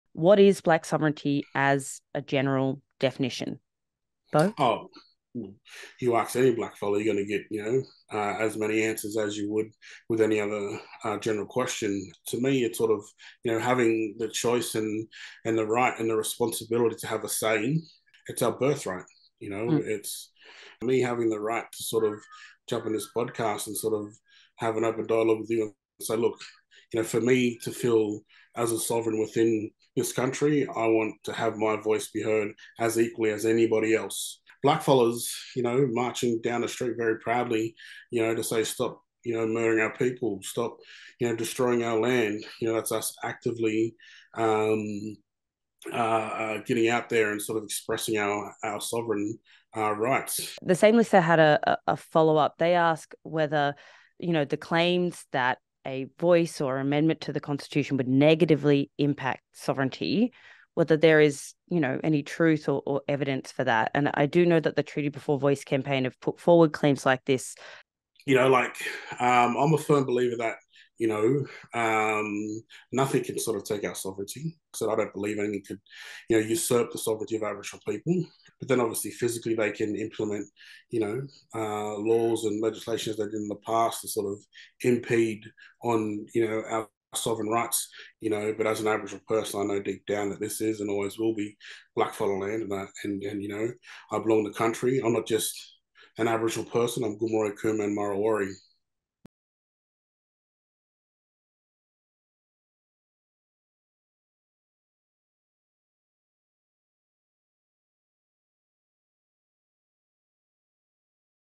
Indigenous voice referendum AMA: What is Blak sovereignty?
First Nations sovereignty movements in Australia have been building for decades but the most recent iteration has been the ‘Blak sovereignty’ movement that includes Senator Lidia Thorpe.

Listeners have asked for a general definition of Blak sovereignty and whether inclusion in the constitution will negatively impact the sovereignty of First Nations people. Boe Spearim, Gamilaraay, Kooma and Murrawari man and convenor of grassroots activist group Treaty Before Voice, discusses what sovereignty and expressing sovereign rights means to him